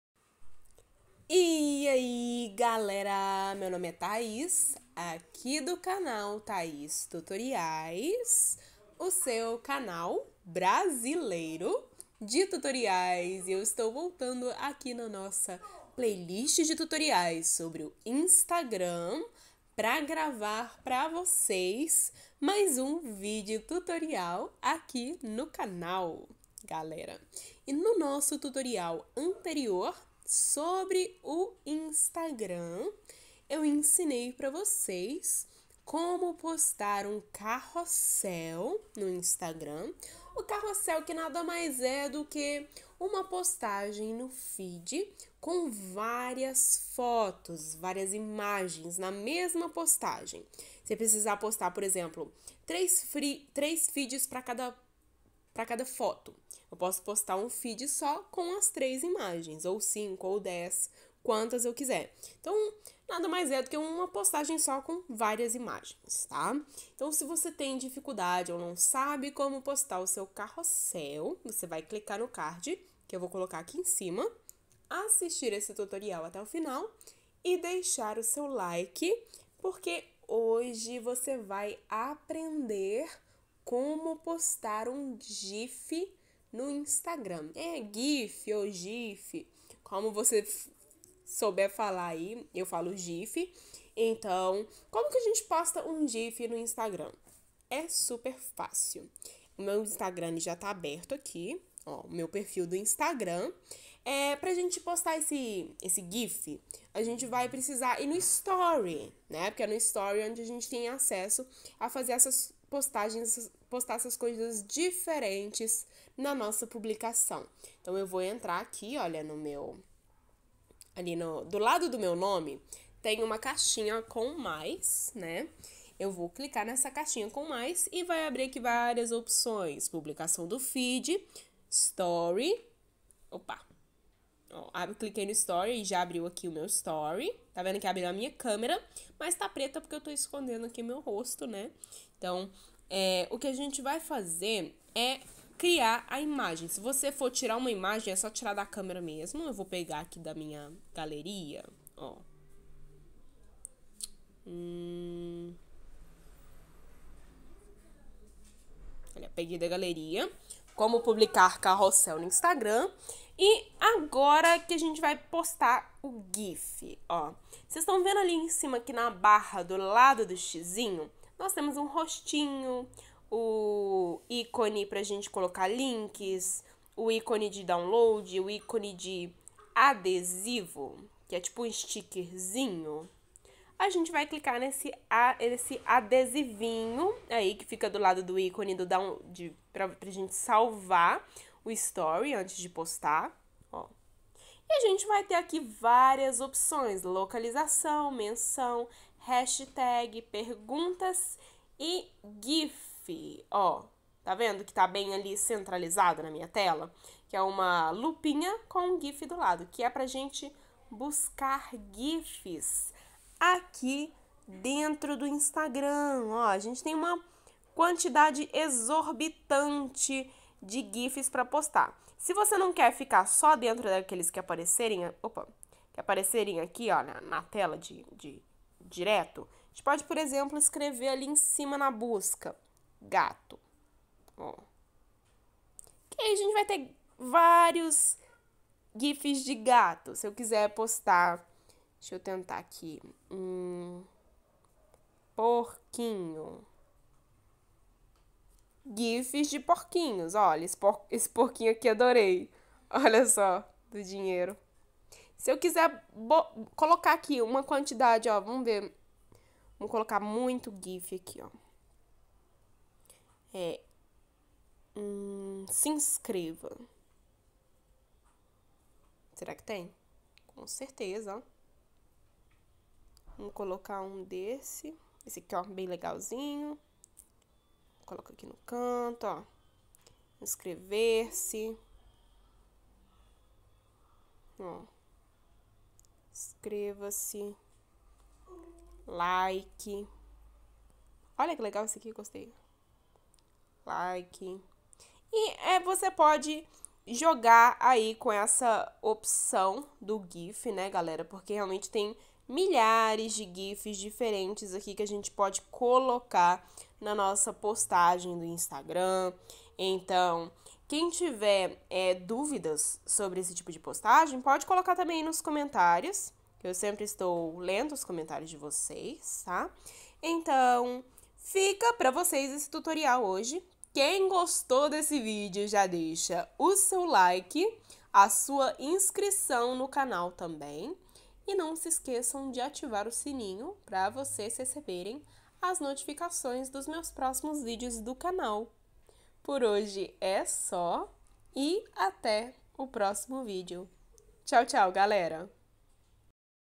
COMO POSTAR GIF NO INSTAGRAM
COMO POSTAR GIF NO INSTAGRAM, este vídeo é um tutorial para você aprender facilmente como postar GIF no INSTAGRAM,